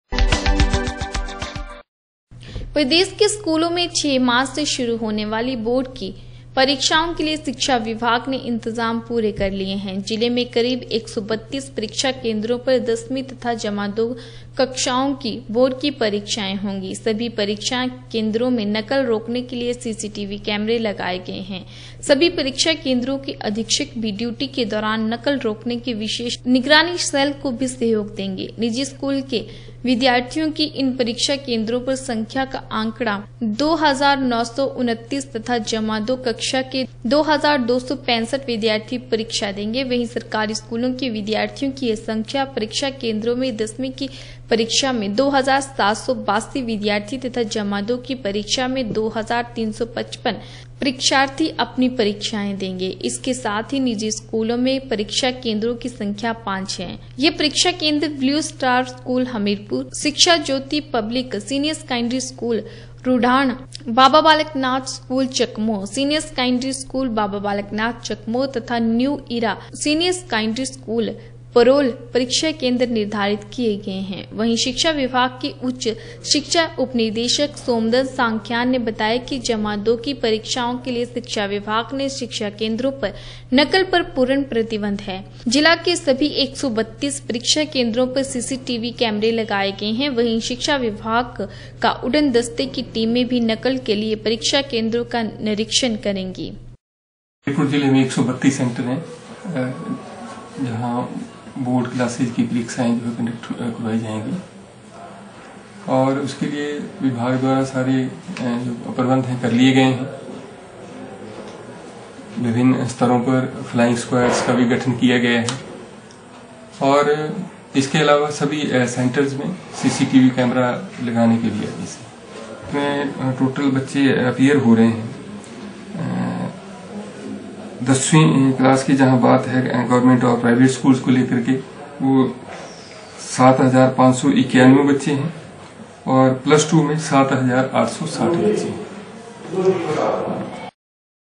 प्रदेश के स्कूलों में शुरू होने वाली बोर्ड की परीक्षाओं के लिए शिक्षा विभाग ने इंतजाम किए पुरे
प्रदेश के स्कूलों में छह मार्च से शुरू होने वाली बोर्ड की परीक्षाओं के लिए शिक्षा विभाग ने इंतजाम पूरे कर लिए हैं। जिले में करीब 132 परीक्षा केंद्रों पर दसवीं व जमा दो कक्षाओं की बोर्ड की परीक्षाएं होंगी। सभी परीक्षा केंद्रों में नकल रोकने के लिए सीसीवीटी कैमरे लगाए गए हैं। सभी परीक्षा केंद्रों के अधीक्षक भी ड्यूटी के दौरान नकल रोकने के विशेष निगरानी सेल को भी सहयोग देंगे।